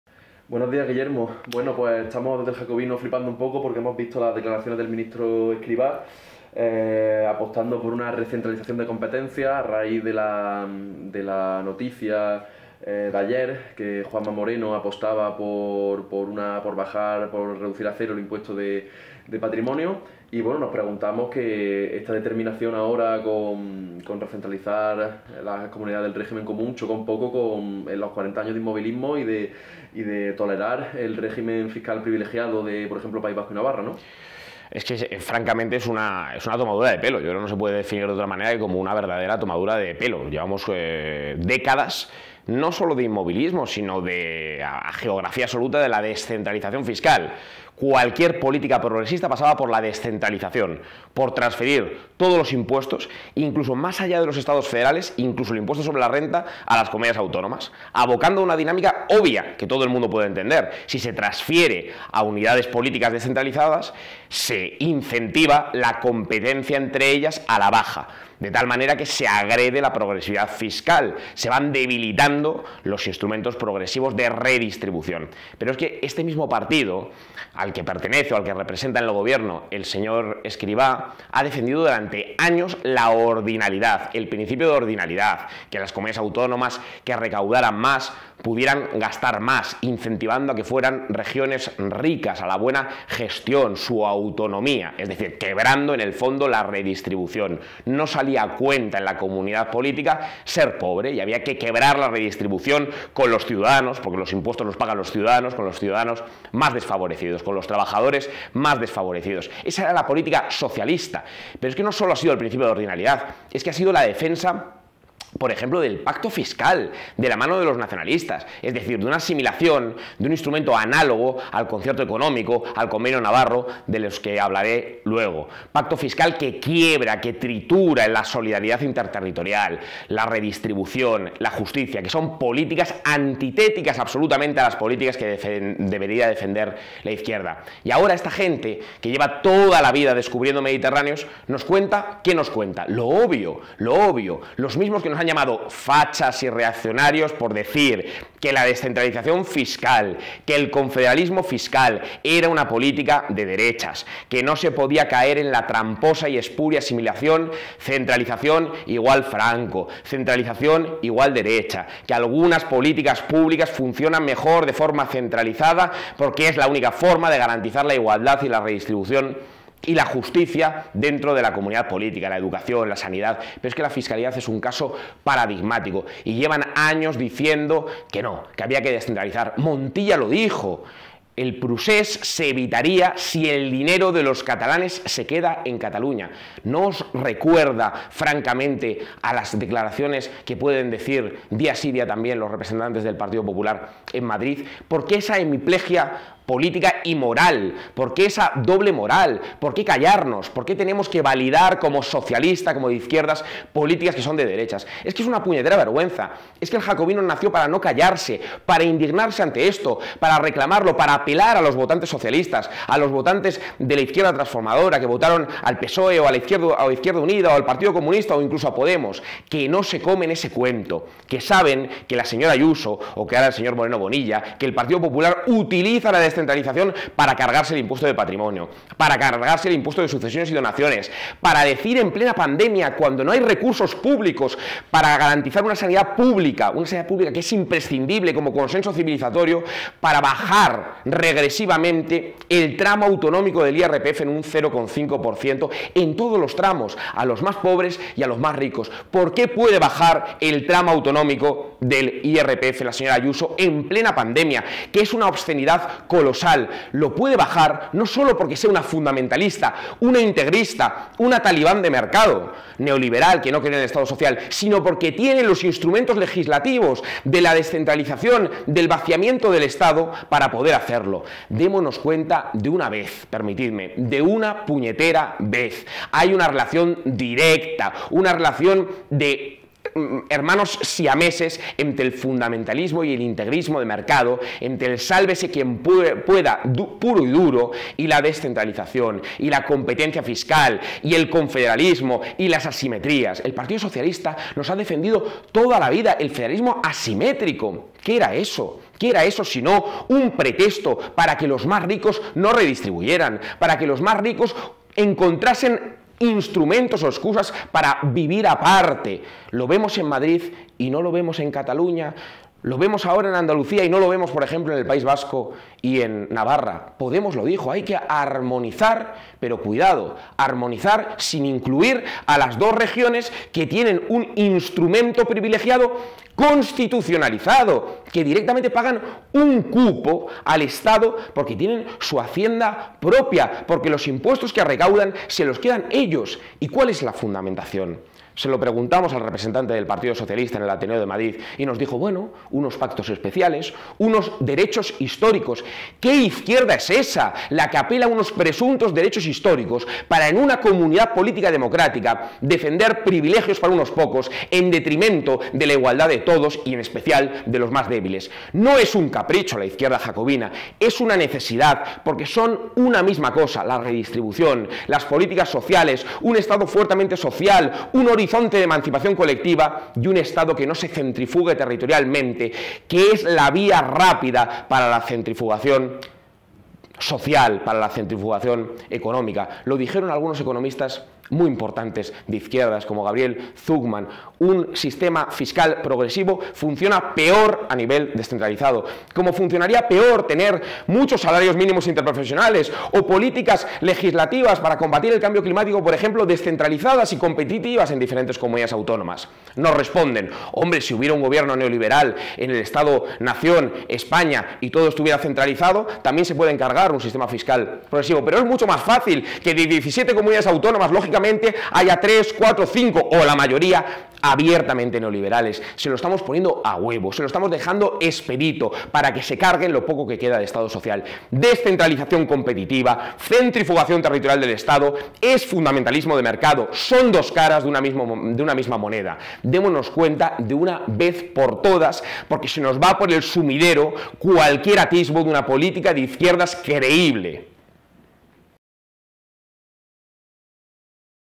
🗣 Guillermo del Valle REACCIONA a las declaraciones de ESCRIVÁ sobre la RECENTRALIZACIÓN FISCAL.
Sólo El Jacobino tiene una posición coherente con la igualdad. Hay una relación directa entre el fundamentalismo de mercado y la descentralización competitiva. Señor Escrivá, dejen de tomarnos el pelo.

🌟 ¡AYÚDANOS! Entra a y colabora con tus donaciones para que El Jacobino pueda seguir realizando su labor. ¡ES MUY IMPORTANTE!